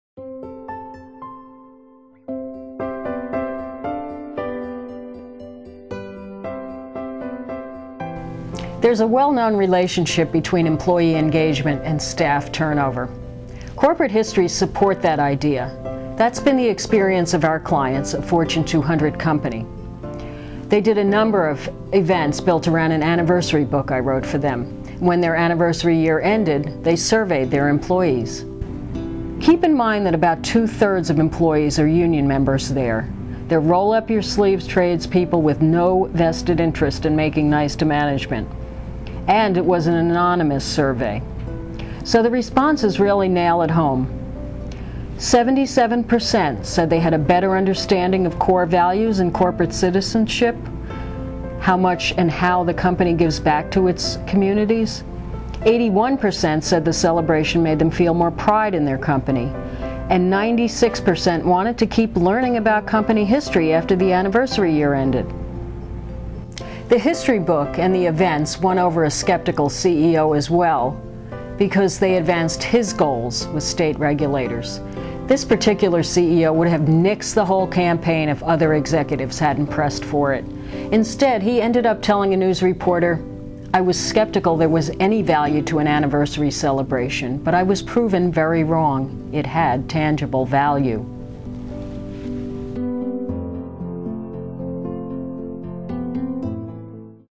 Making the Business Case
Employees feel more engaged at work when they learn their company's history. That's what our Fortune 200 client discovered when it surveyed its 10,000 workers after a year-long 100th anniversary campaign. (Because of confidentiality agreements, we can't publicize the client's name here.) Two-thirds of the workers are unionized, and you can be sure they told it like it was. The vast majority had more pride and a better understanding of the business.